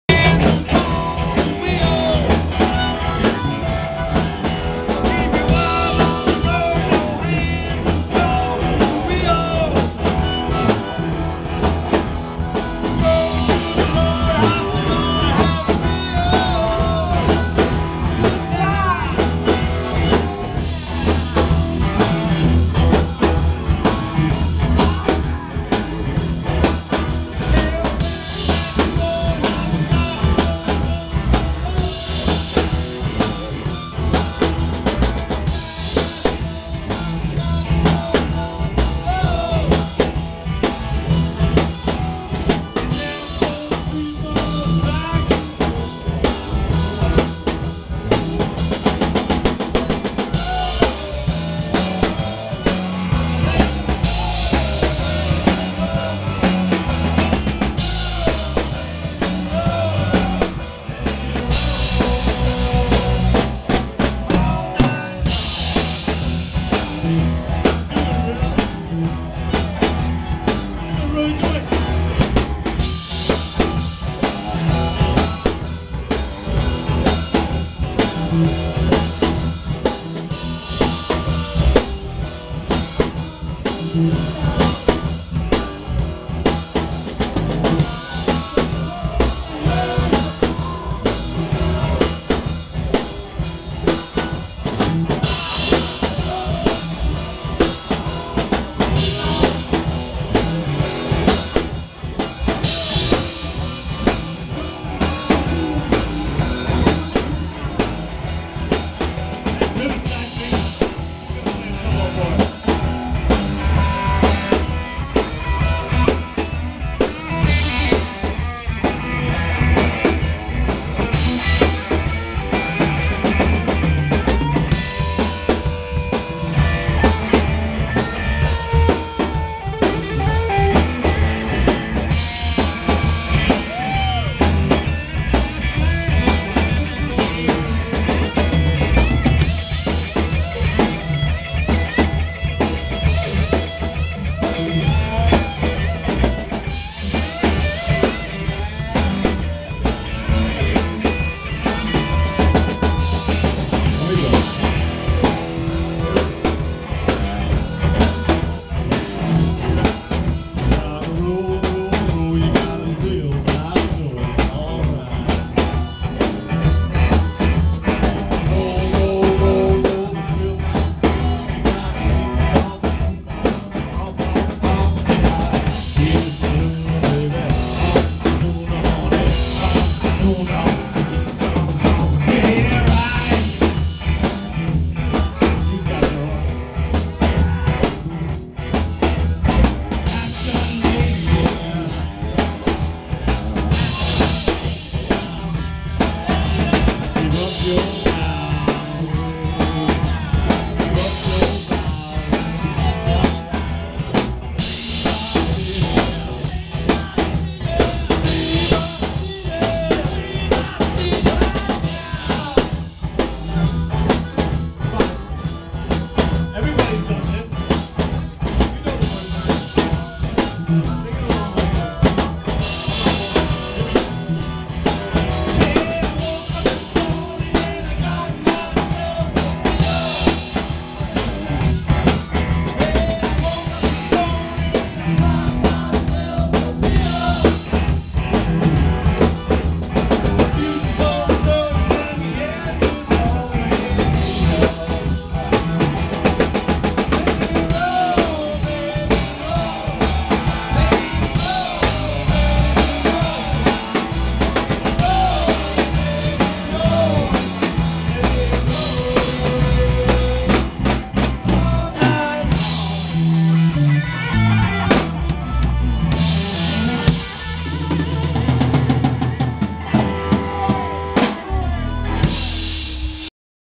Open jam with the Voo Doo Doctors
OPEN jam with Voo Doo Doctors. Riverside Vinny on guitar. Chris Bergeron on bass, Gerry Milot on drums.